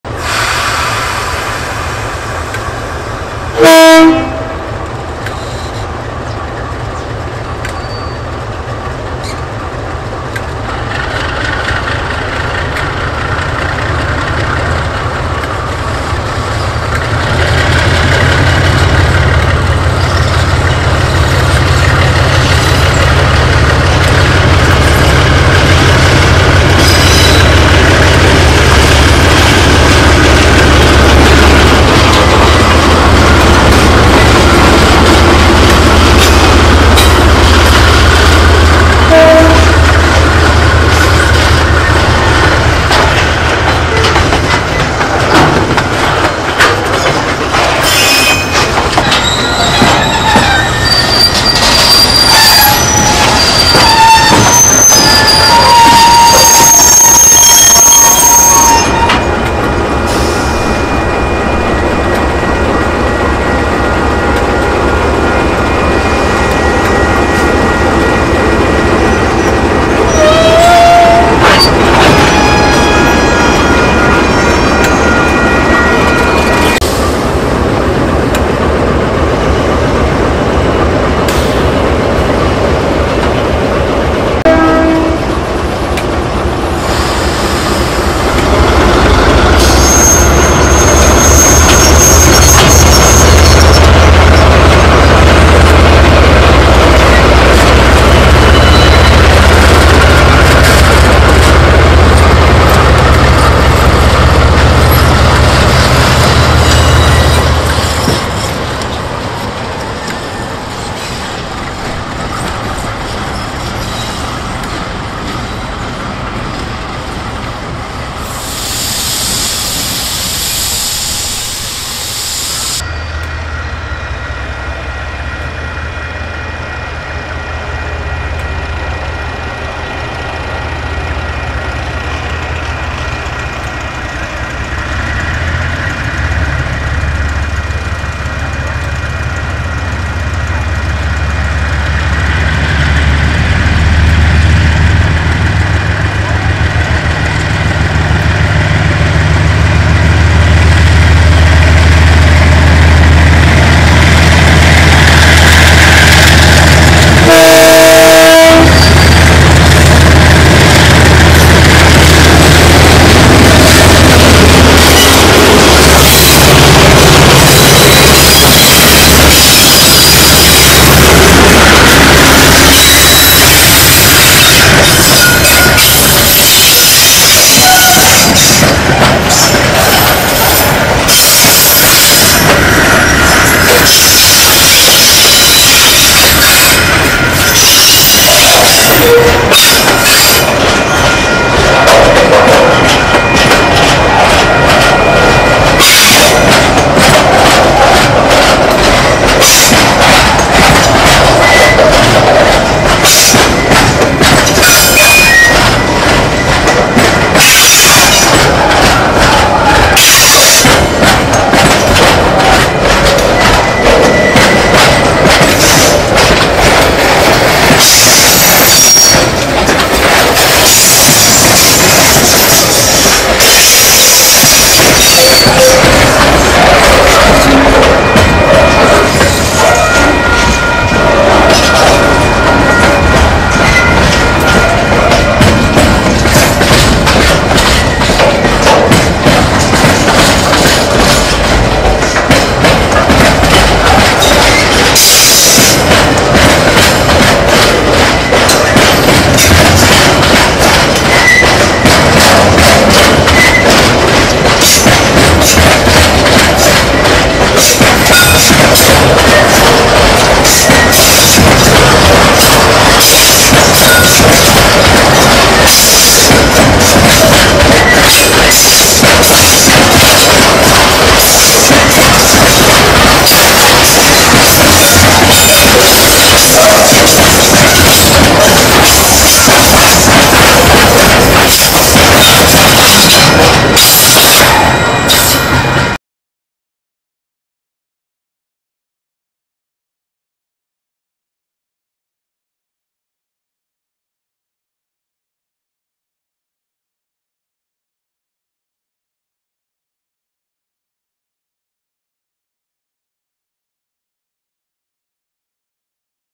KiwiRail Locomotives DXB 5448 and DXB 5137 Collecting Freight Wagons in Edendale
KiwiRail Locomotives DXB 5448 and DXB 5137 Collecting Freight Wagons in Edendale, in the Southland Region of New Zealand's South Island.